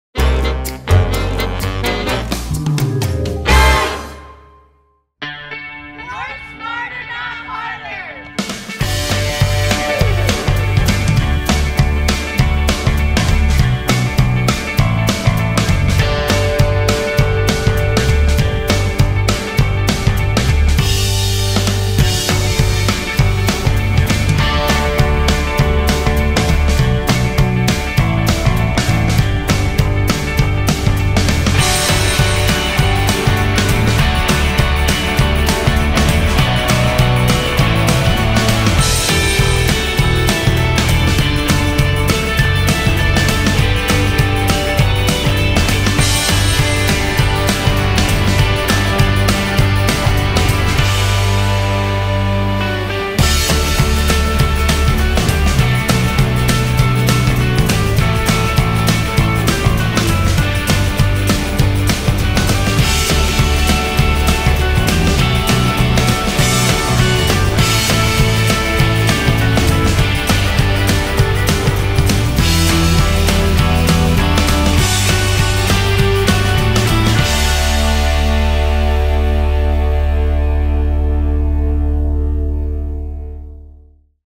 Work Smarter Not Harder Summer School 2018
Video created by the students in Ms. Canga's class about how to do school work better!